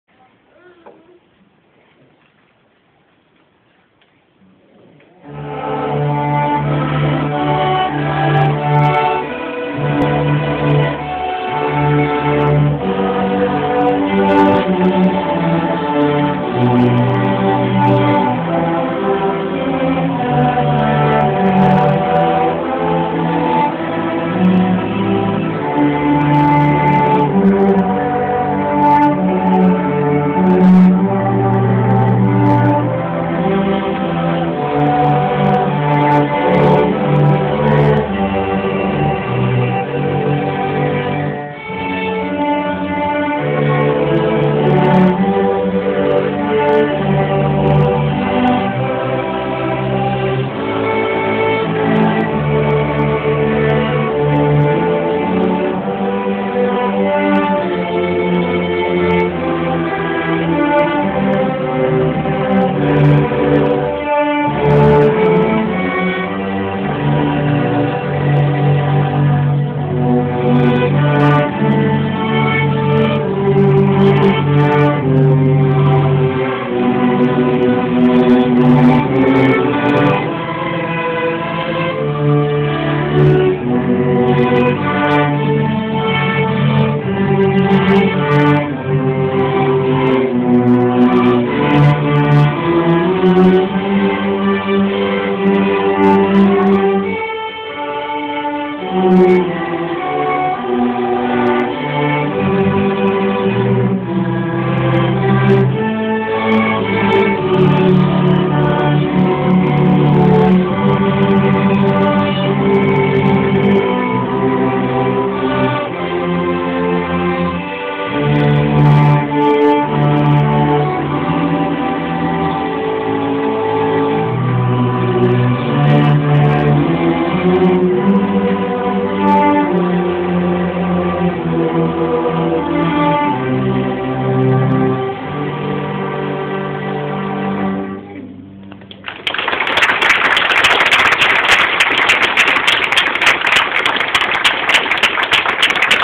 String Orchestra Concert Spring 2009 Pt. 10
This is the tenth and last video in my series on the spring concert of my daughter's string orchestra group. They have improved a lot over the last three years.